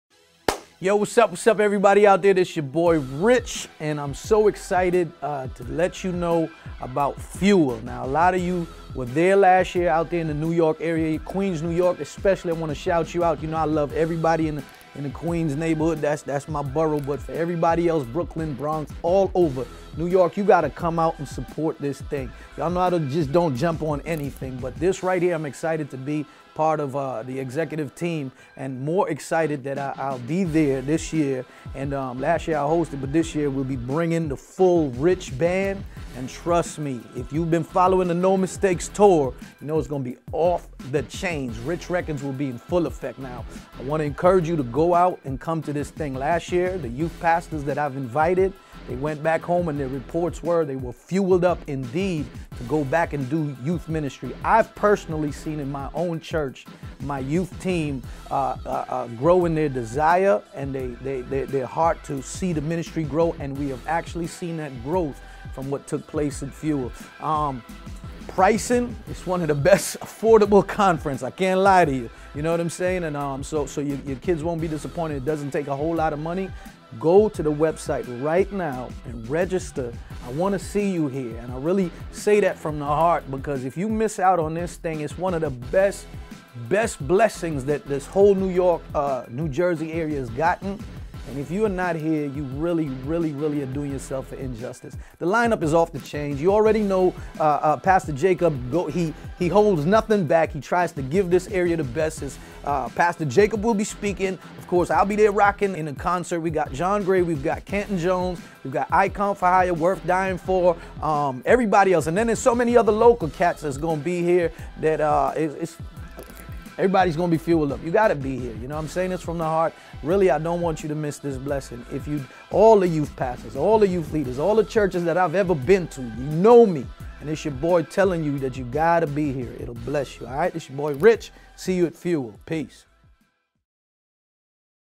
Richie Righteous talks about FUEL STUDENT CONFERENCE
Fuel Student Conference takes place on October 5-7th. to register today. Don't miss it.